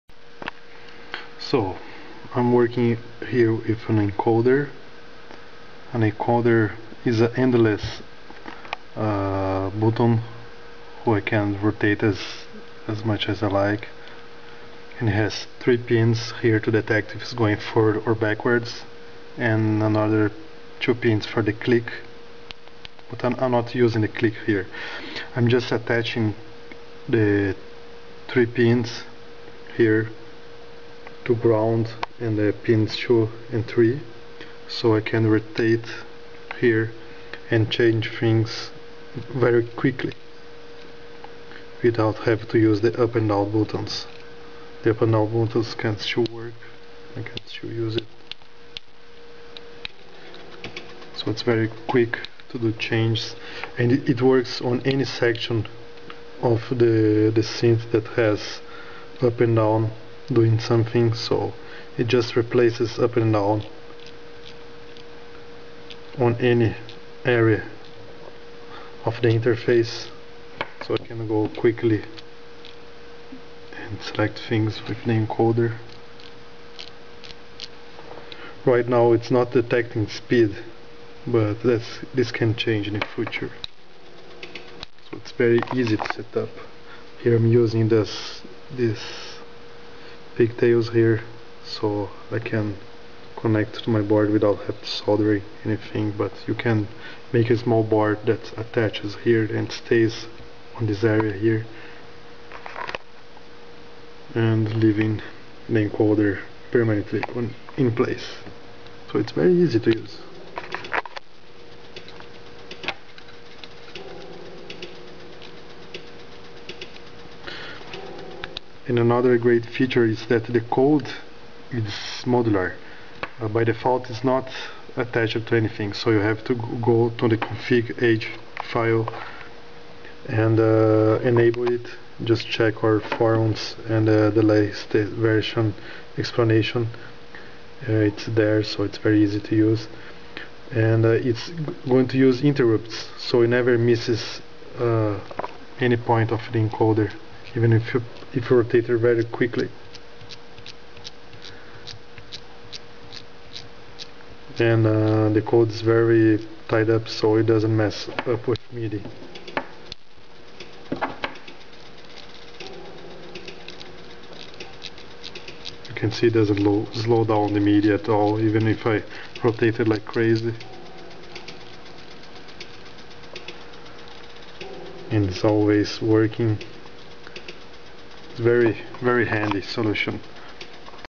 Beat707 Hack #4 - Endless Encoder Input
Hacks and Mods # 004 - Endless Encoder Input

Edit: We have updated the software with the following: ENCODER_SPEED added to Config.h - When set, it will detect the speed of the encoder movement and apply changes faster or slower